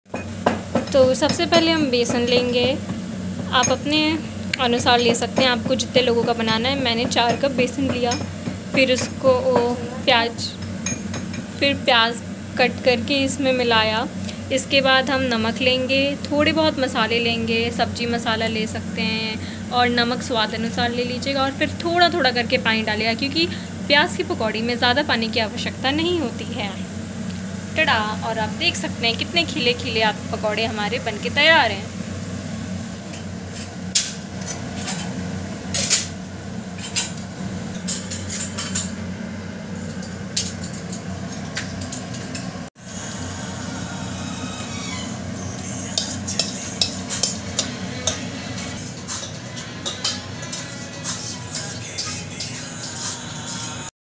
How to make Basan ke pakaudi बारिश में बेसन और प्याज़ के पकौड़ी के मजे
बेसन और प्याज़ के पकौड़ी बनाने की रेसिपी